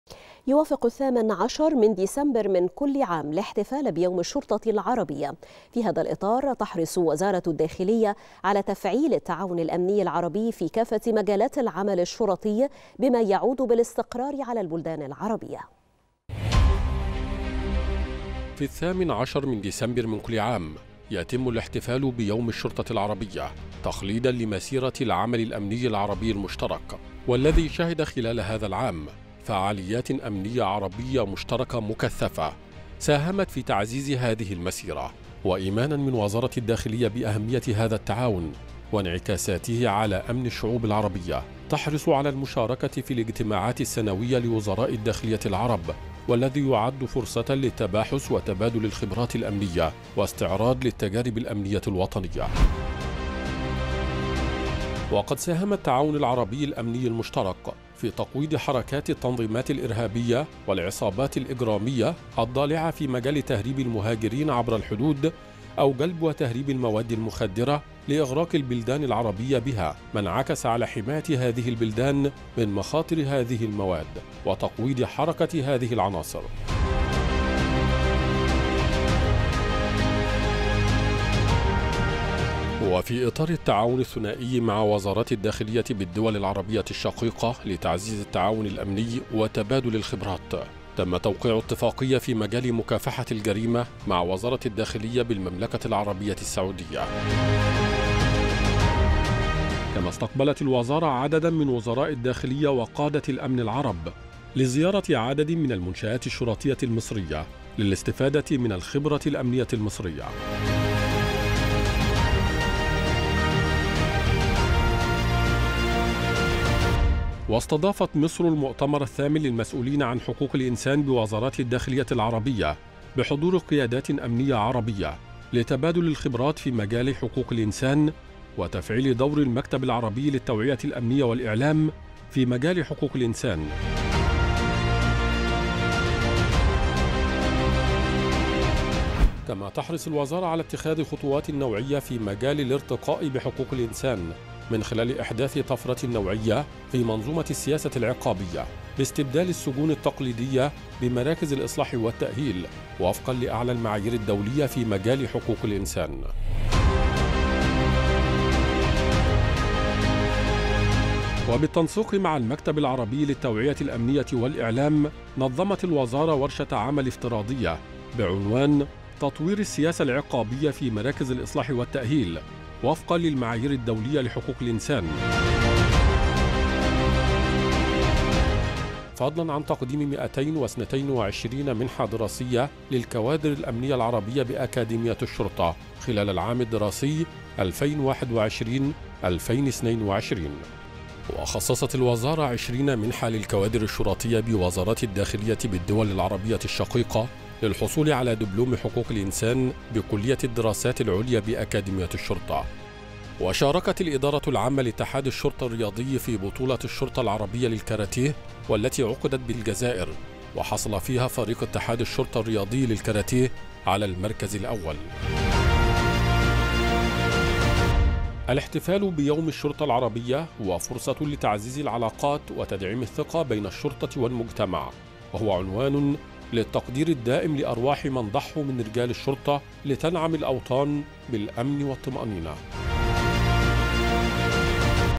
وزارة الداخلية تشارك في الاحتفال بيوم الشرطة العربية للتأكيد على التعاون بين مصر وأشقائها العرب
نشرة التاسعة | وزارة الداخلية تشارك في الاحتفال بيوم الشرطة العربية للتأكيد على التعاون بين مصر وأشقائها العرب